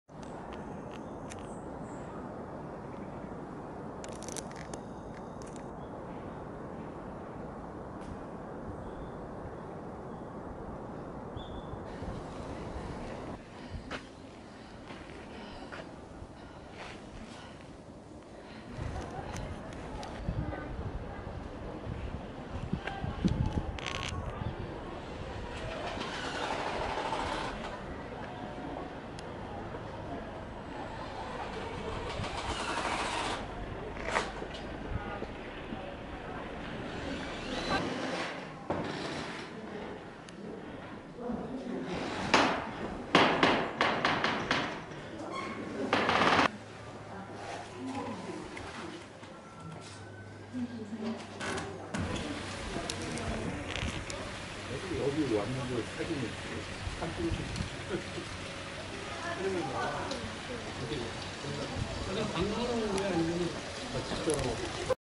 神戸北野異人館
2023/02/22
大龍寺から下山するコースで初めて北野の異人館の北側へ下りました。急勾配でウロコの館の裏側につきました。
さらに歩くと北野天満宮の境内へと繋がっていました。
風見鶏と萌葱の館も見学。65才以上の神戸市民は無料です。